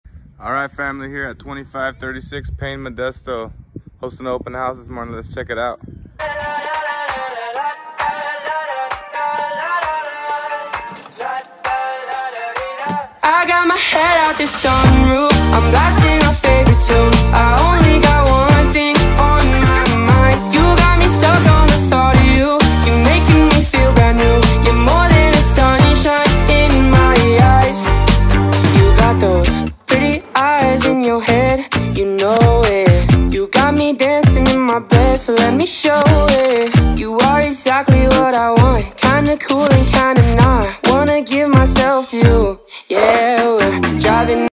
House in Modesto CA￼
2536 Payne ave modesto
Listing Price 565,000
4 bed 3 bath
2061 sq.ft
Exit Realty Consultants
DRE# 02179676